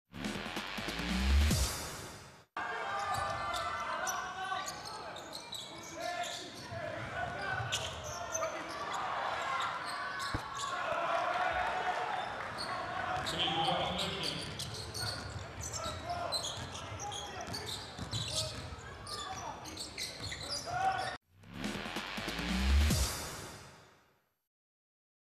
Genius assist by Kenan Kamenjaš (Borac Mozzart – SC Derby, 10.11.2023)
Kenan Kamenjaš came up with this genius assist in the game of his SC Derby against Borac Mozzart in Borac Sports Hall, Čačak. Check it out!